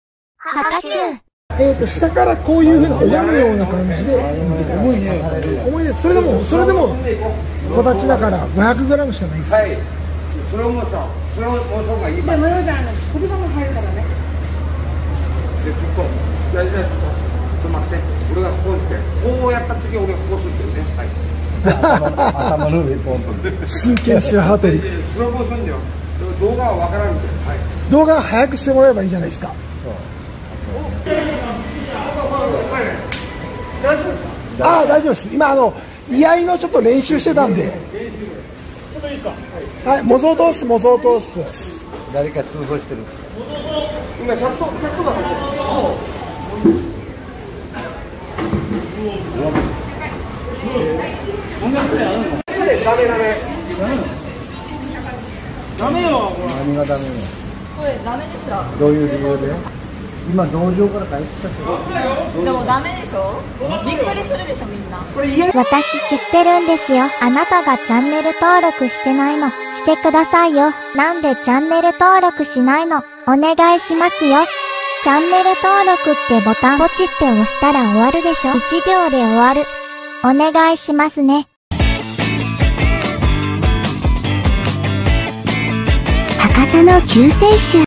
【悲劇】うめみつ が誕生日プレゼントで貰った模擬刀を天神のど真ん中で振った歯がないオッサンの末路#珍百景#面白い動画#警察
【編集後記】

今回行った場所の紹介👇️

【天神ライオン広場】